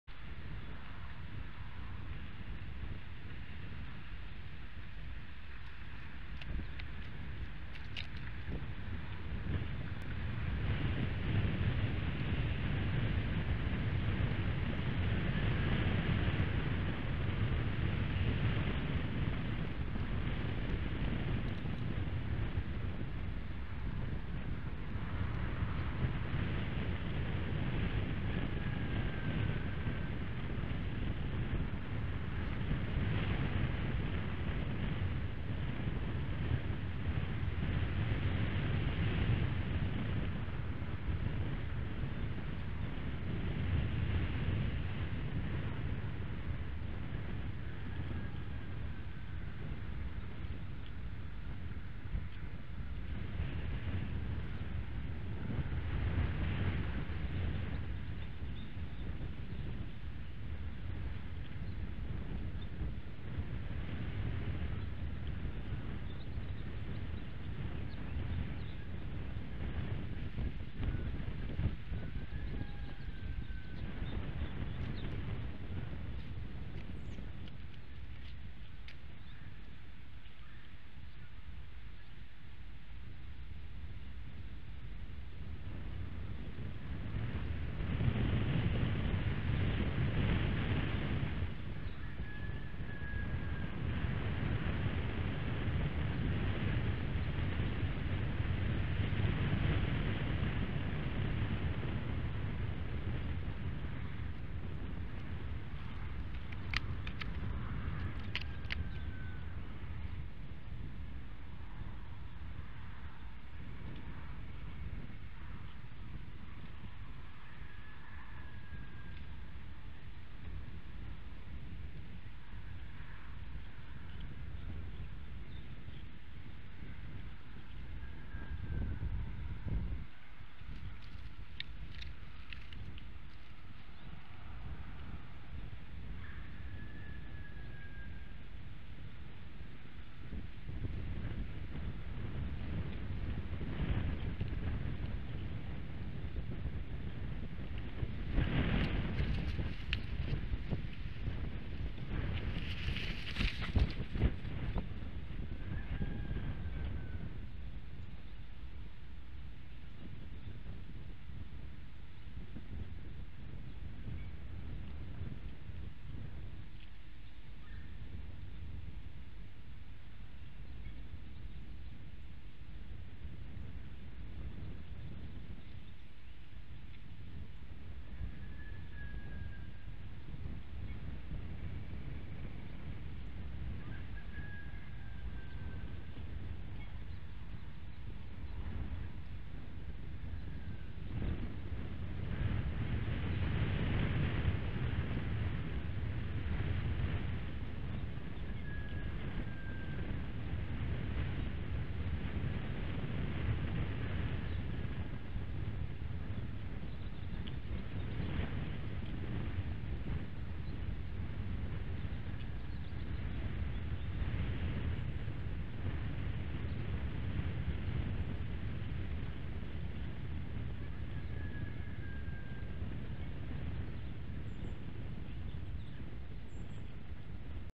Still active til tree sitting 2 12 18 Also in the 7 30ish time frame local time
Playing in the nest until flying north and sitting in a tree.